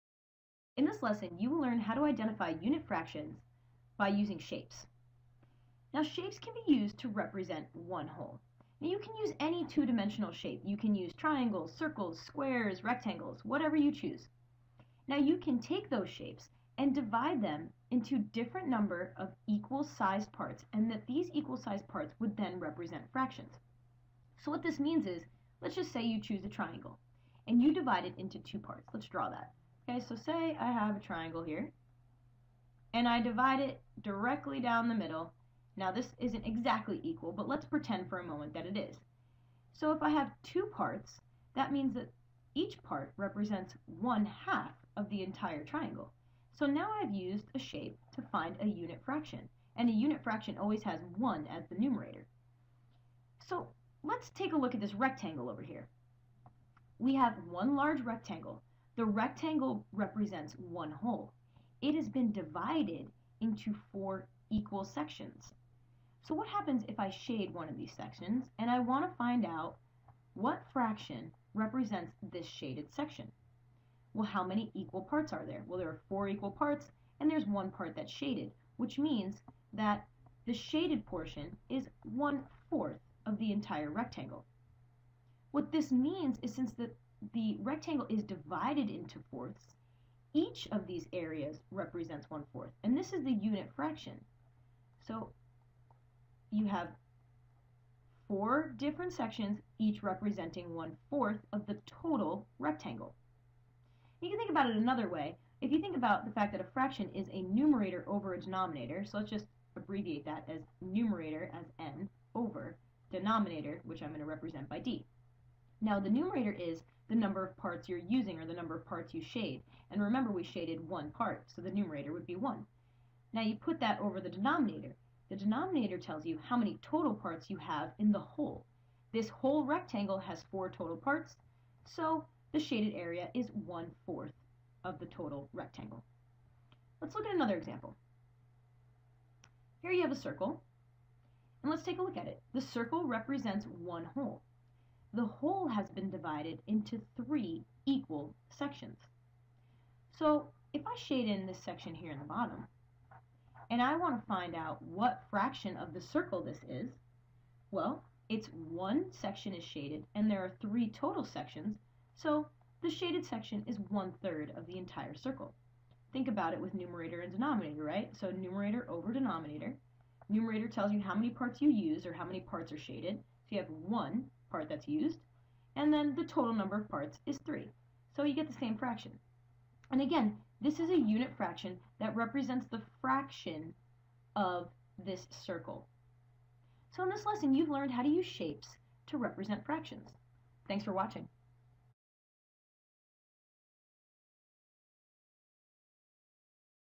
Identifying Unit Fractions of Partitioned Shapes: 3.G.1
This lesson shows shapes that are partitioned into unit fractions. The fraction is identified and described using words.